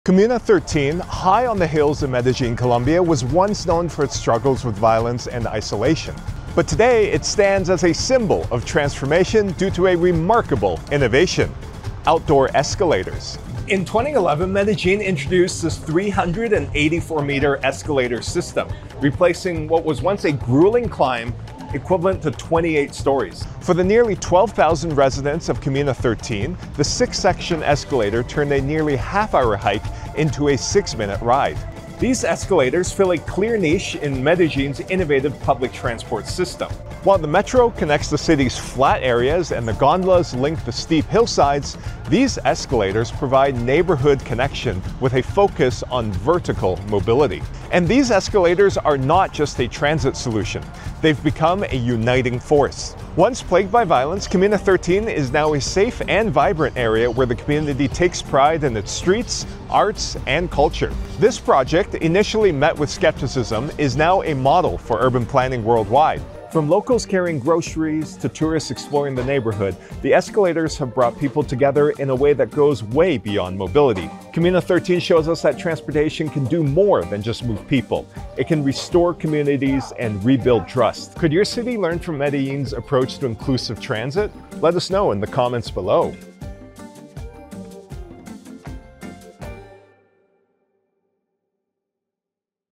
Medellín's outdoor escalators connect communities 💖🏙️
Ever imagined turning a 30-minute hike into a 6-minute ride? 🚶‍♂️

*Medellín’s outdoor escalators* are doing just that! *George Liu, Senior Researcher at Technical University of Munich,* explains this innovative system. He details how it's transforming how people get around, while restoring communities, rebuilding trust, and bringing neighbourhoods together. 🌆💖